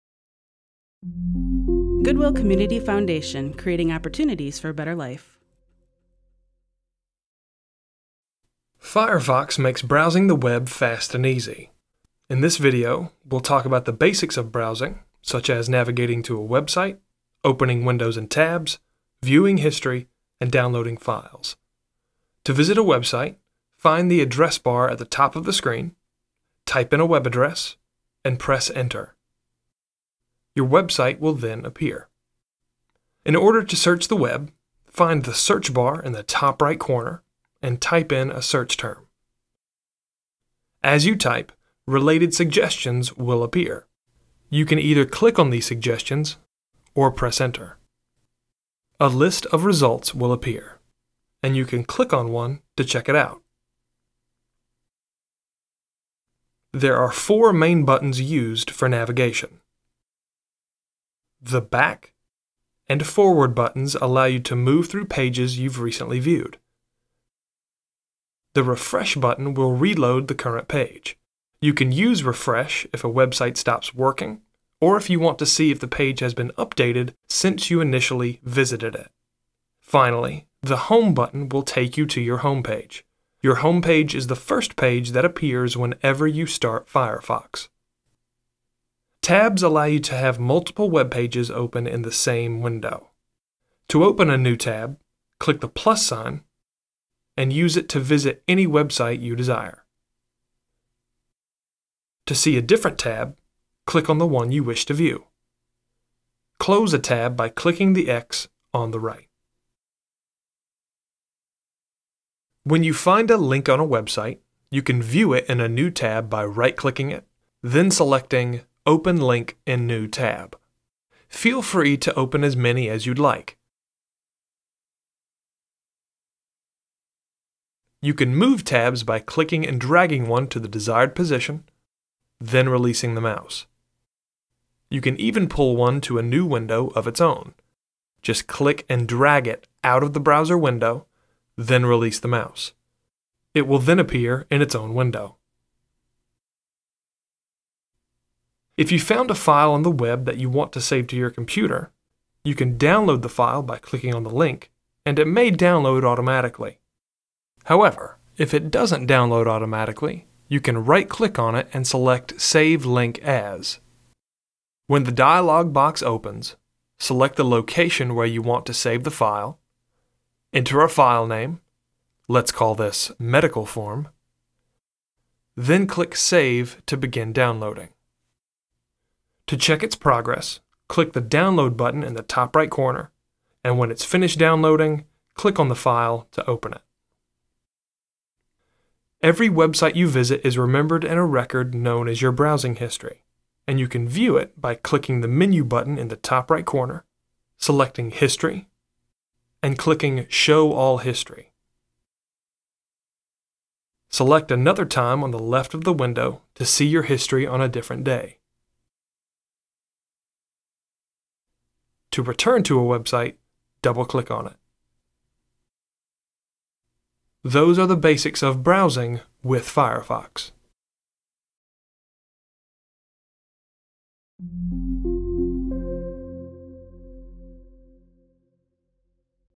Firefox: Browsing with Firefox
In this video, you’ll learn more about browsing in Firefox. for our text-based lesson.

This video includes information on:
• Managing windows and tabs
• Viewing browsing history
• Downloading and accessing files